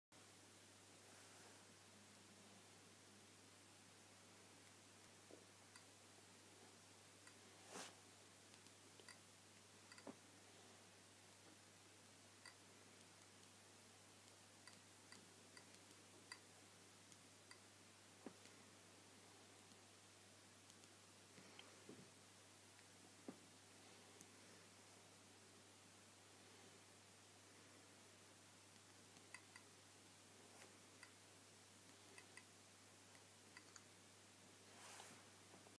Galaxy Nexus vs Nexus 4 PIN unlock
strange lag issues with Nexus 4 PIN unlock